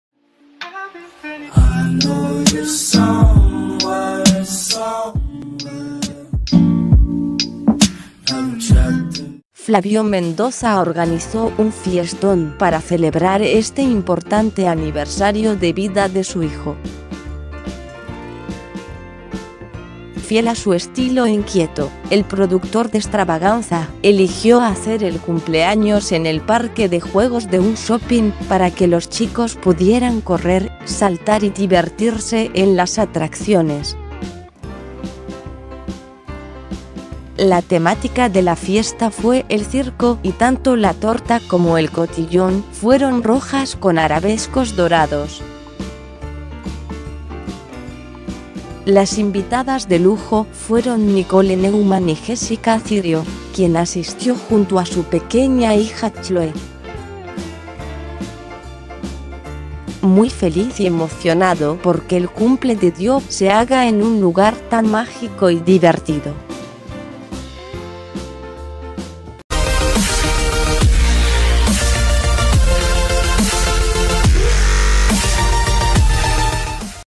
Las postales más tiernas del primer cumpleaños de Dionisio
Flavio Mendoza organizó un fiestón para celebrar este importante aniversario de vida de su hijo.

Fiel a su estilo inquieto, el productor de "Stravaganza" eligió hacer el cumpleaños en el parque de juegos de un shopping para que los chicos pudieran correr, saltar y divertirse en las atracciones.

La temática de la fiesta fue "el circo" y tanto la torta como el cotillón fueron rojas con arabescos dorados.

Las invitadas de lujo fueron Nicole Neumann y Jésica Cirio, quien asistió junto a su pequeña hija Chloé.

"Muy feliz y emocionado por que el cumple de Dio se haga en un lugar tan mágico y divertido. ¡Quiero que sea inolvidable!", escribió emocionado Flavio.

¡Mirá las fotos!